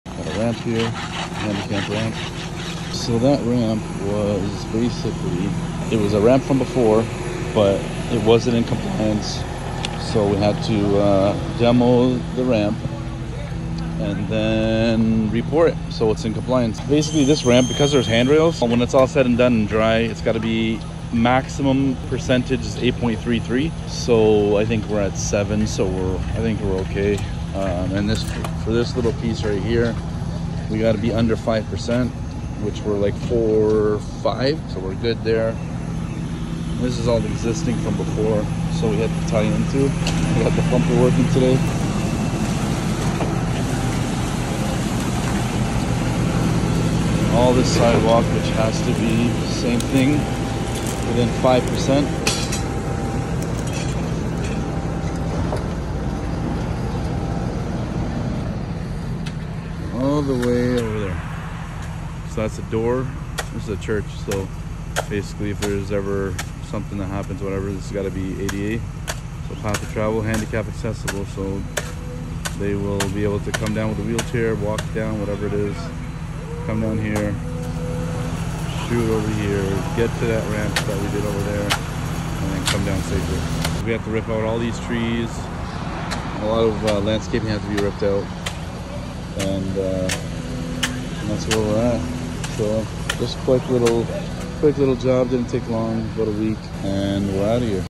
ADA RAMP & SIDEWALK
Another successful commercial concrete project in Costa Mesa, CA - This time we built a fully ADA-compliant ramp and sidewalk. This job consisted from grading, forming to pouring and finishing, every step was done with precision to meet accessibility standards and local code. Our team takes pride in delivering quality concrete work that's built to last and look great.